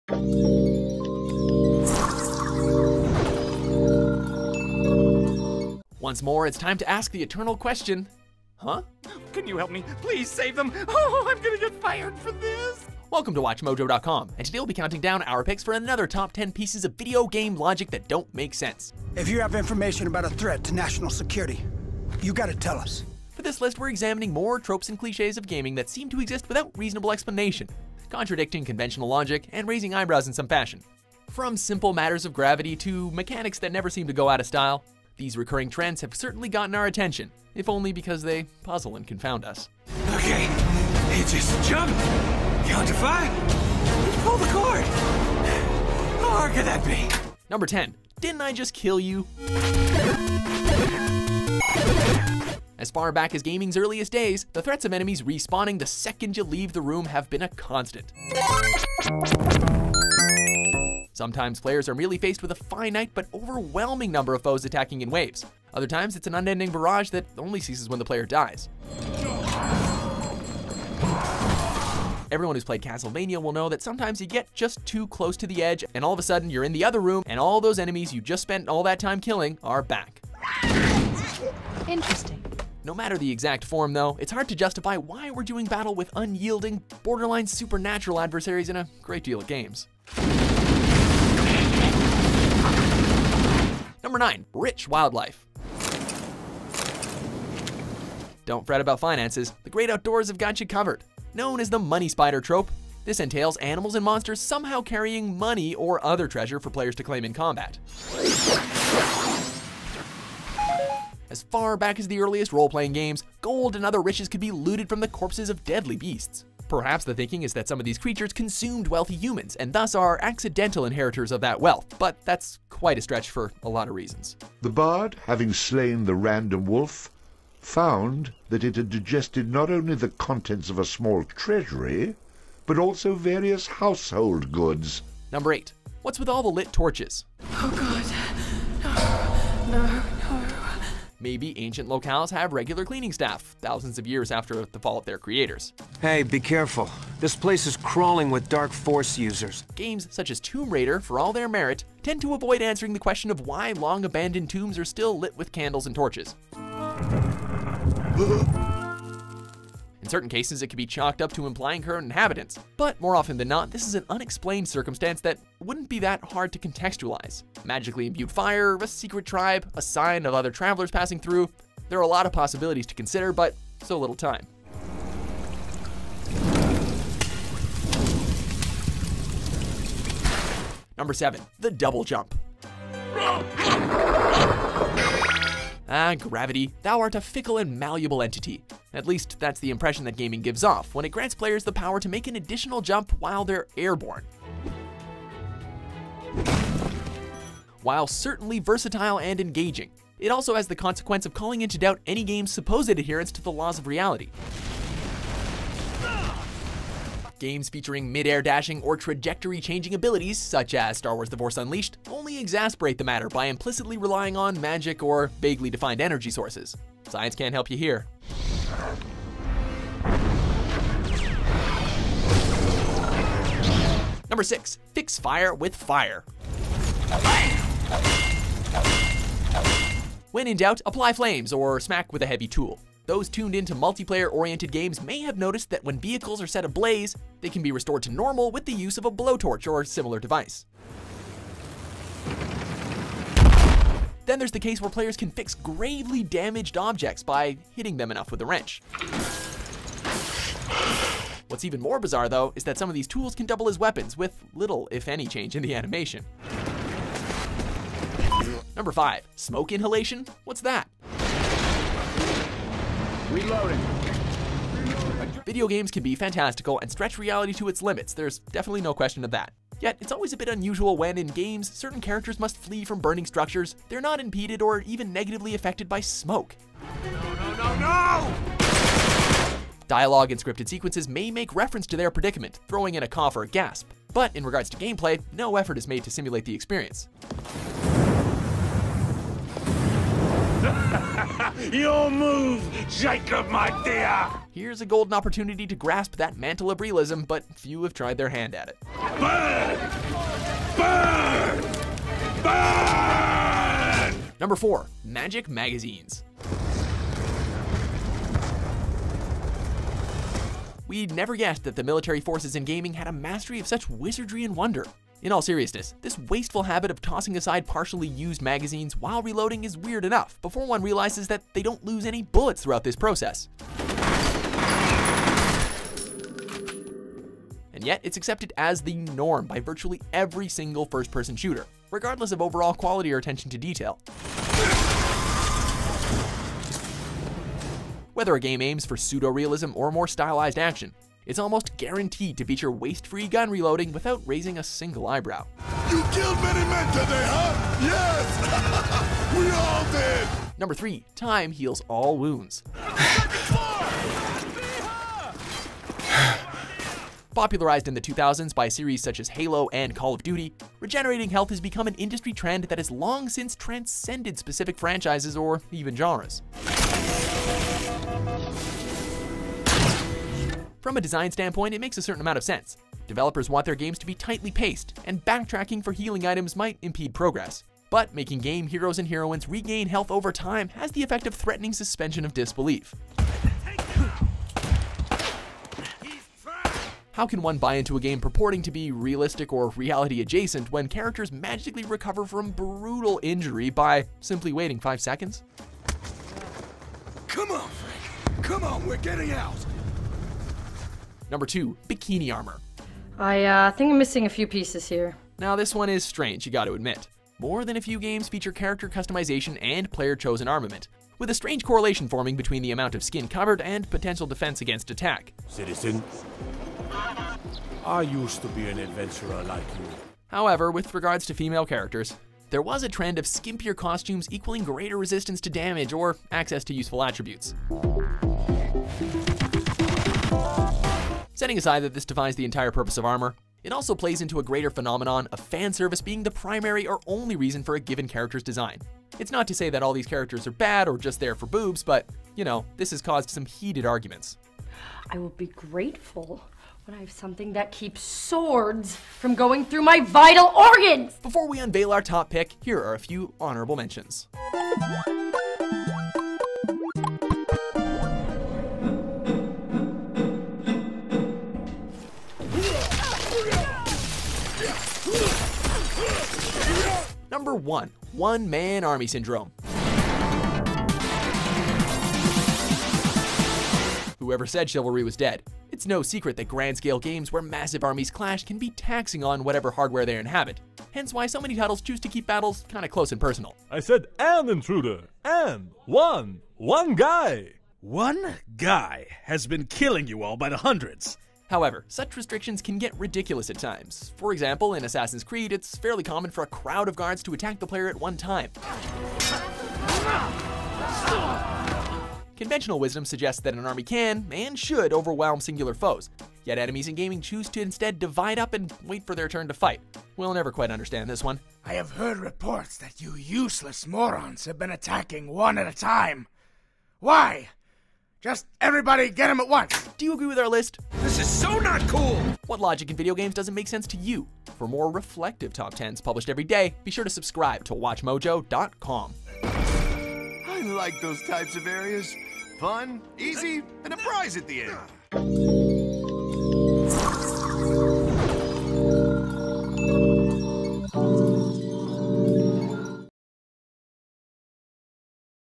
Another Top 10 Video Game Logics That Don't Make Sense
Once more it's time to ask the eternal question, “Why are these things in the games we love?” as we count down our picks for the Another Top 10 Video Game Logic That Doesn't Make Sense.

For this list we're examining more tropes and cliches of gaming that seem to exist without reasonable explanation, contradicting conventional logic and raising eyebrows in some fashion.  From simple matters of gravity to more troubling implications related to gender, these recurring trends have certainly gotten our attention – if only because they puzzle and confound us.

Special thanks to our user Norris Vaughn for submitting the idea on our interactive suggestion tool